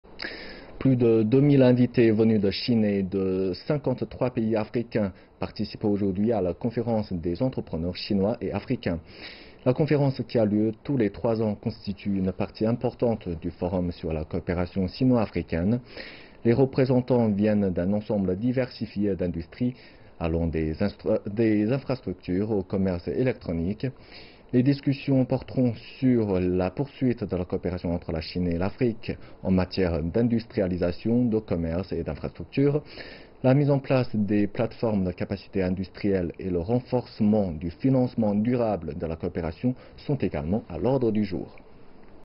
Coopération industrielle, partenariat commercial et infrastructures
Plus de 2 000 invités venus de Chine et de 53 pays africains participent aujourd'hui à la Conférence des entrepreneurs chinois et africains. La conférence qui a lieu tous les 3 ans, constitue une partie importante du Forum sur la coopération sino-africaine. Les représentants viennent d'un ensemble diversifié d'industries, allant des infrastructures, au commerce électronique. Les discussions porteront sur la poursuite de la coopération entre la Chine et l’Afrique en matière d’industrialisation, de commerce et d’infrastructures. La mise en place de plateformes de capacités industrielles et le renforcement du financement durable de la coopération sont également à l'ordre du jour.